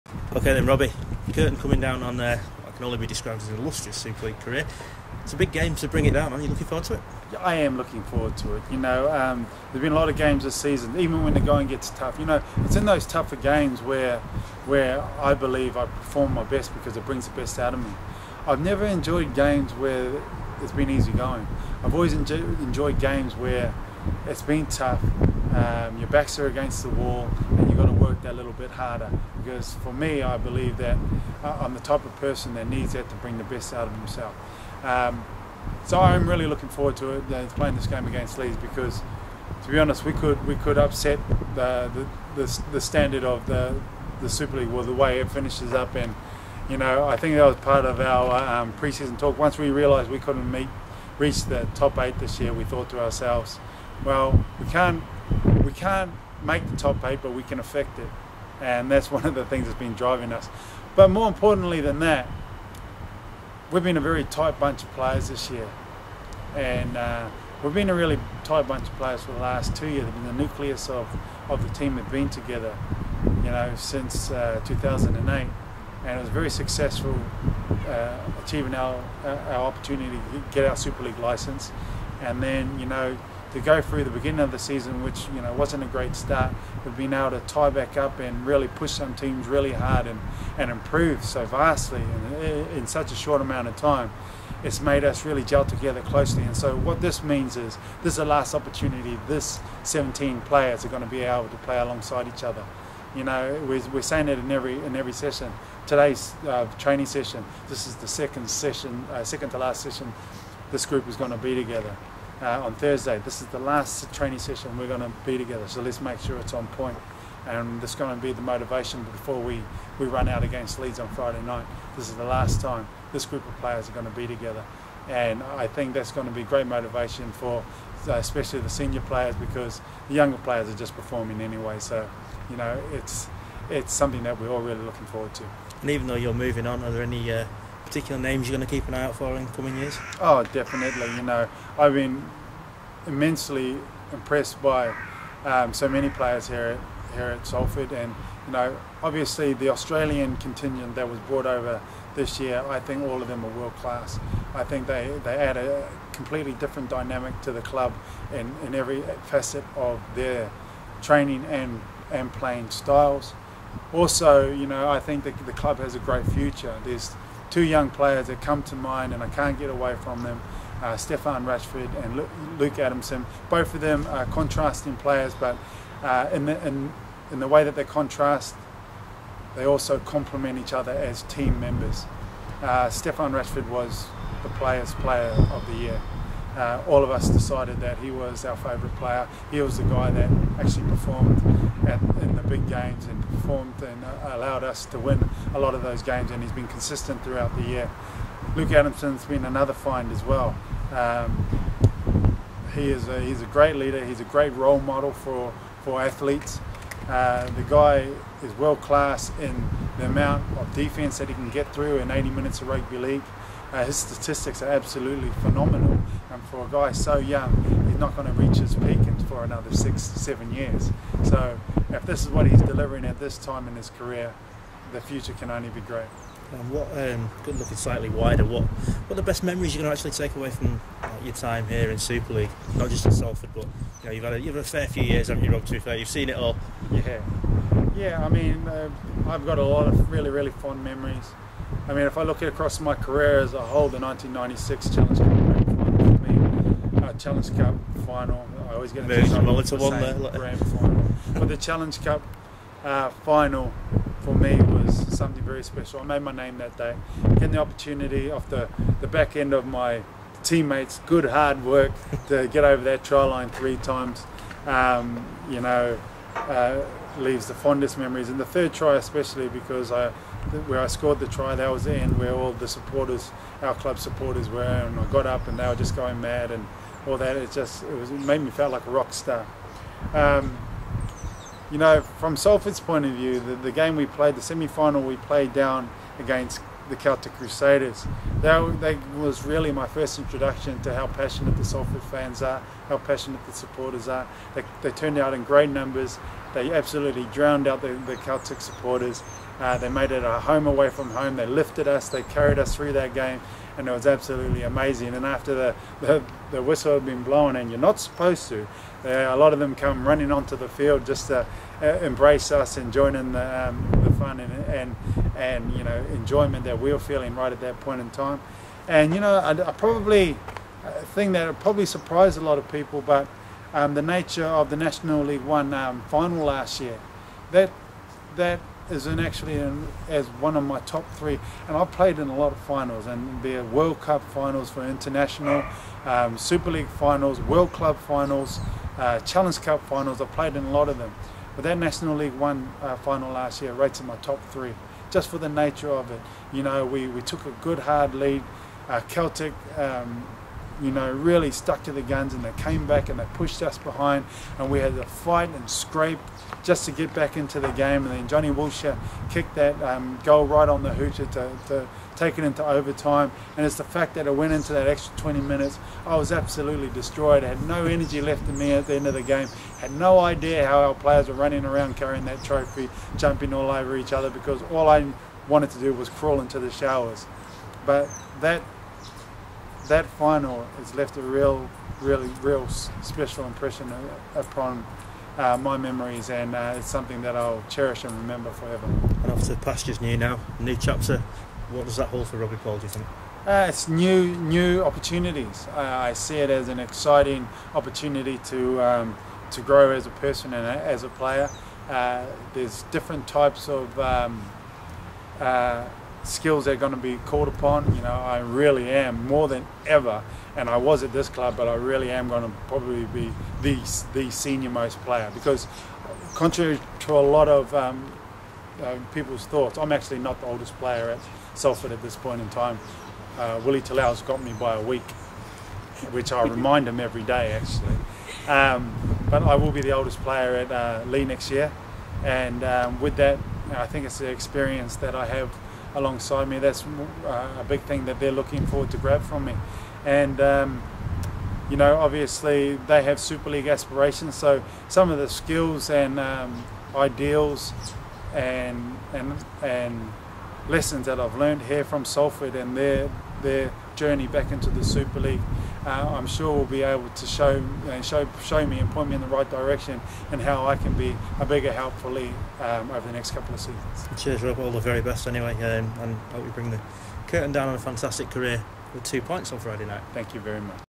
Robbie Paul - Salford City Reds
Robbie is Paul is interviewed by Paul McNally on the eve of his penultimate game in Super League after a career spanning 16 years.